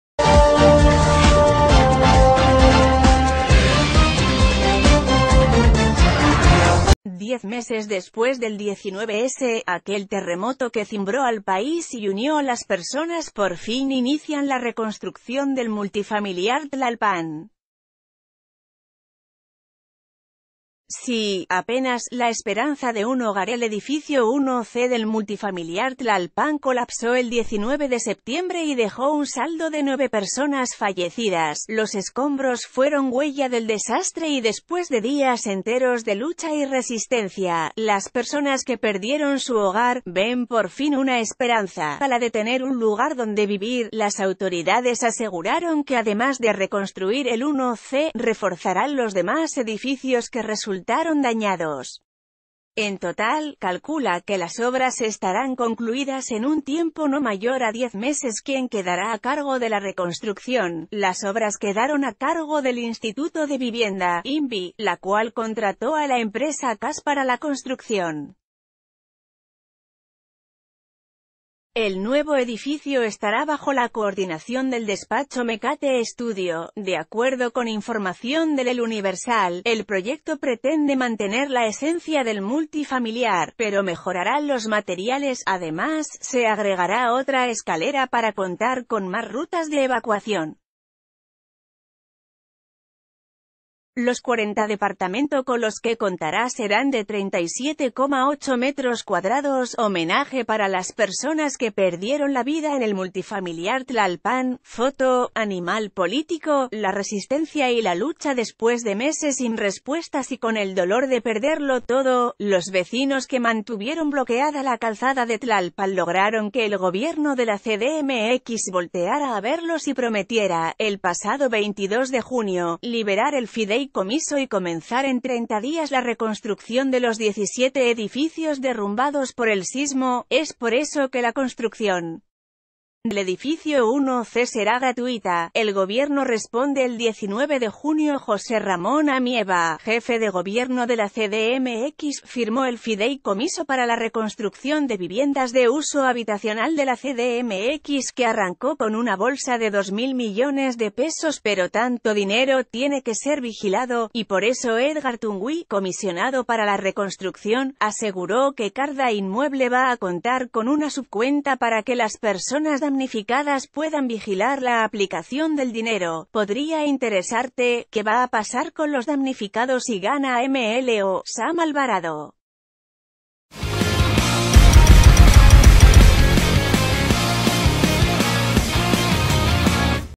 Después de 10 meses de espera, inician reconstrucción del Multifamiliar Tlalpan
10 meses después del 19S, aquel terremoto que cimbró al país y unió a las personas, por fin inician la reconstrucción del Multifamiliar Tlalpan. Sí, apenas. La esperanza de un hogarEl edificio 1C del Multifamiliar Tlalpan colapsó el 19 de septiembre y dejó un saldo de 9 personas fallecidas. Los esco...